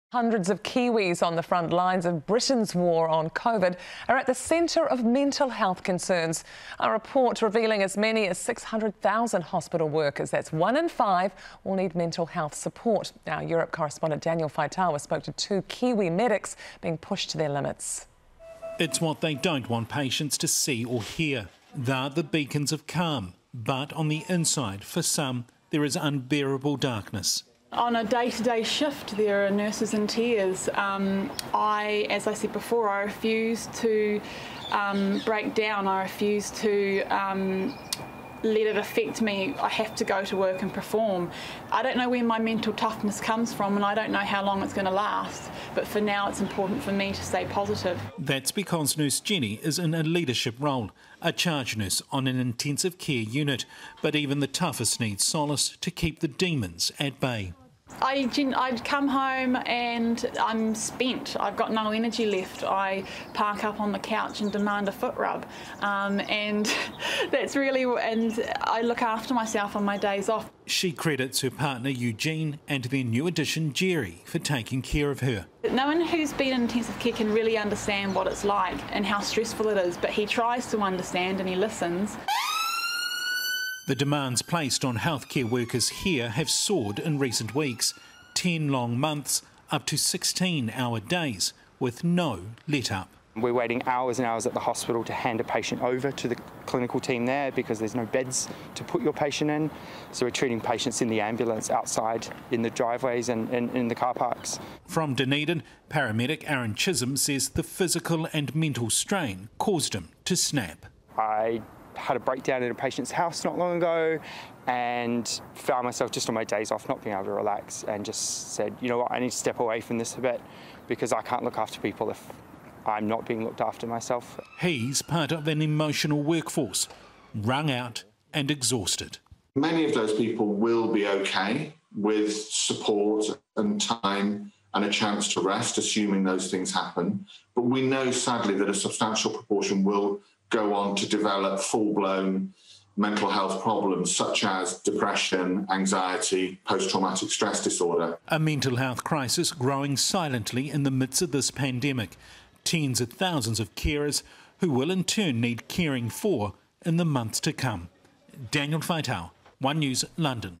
Kiwis on the frontlines of Britain’s war on Covid-19 at the centre of mental health concerns
1 NEWS Europe Correspondent Daniel Faitaua spoke to two Kiwis being pushed to their limits.